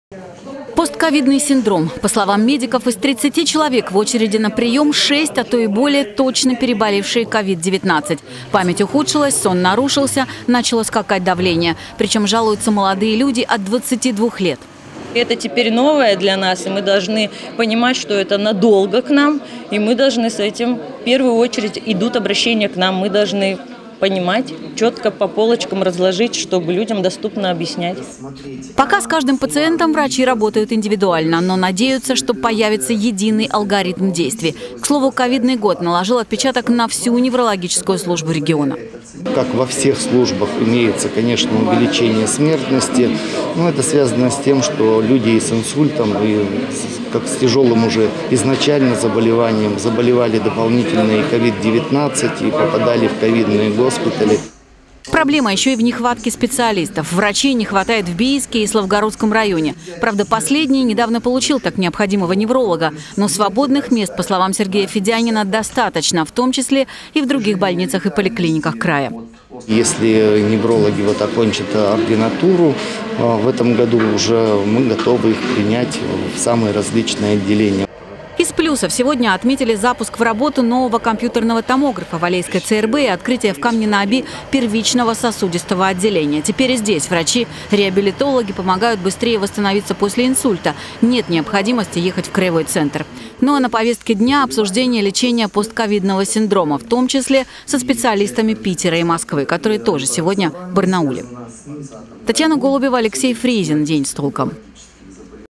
Постcovid-19: Как помочь людям которые переболели COVID-19
Постковидный синдром - то с чем столкнулись неврологи в начале этого года. Врачи говорят - поликлиники буквально переполнены людьми, которые жалуются на депрессию, плохой сон, проблемы с вестибулярным аппаратом. Как помочь этим людям и другие проблемы неврологической службы обсуждали сегодня в рамках краевой научно-практической конференции.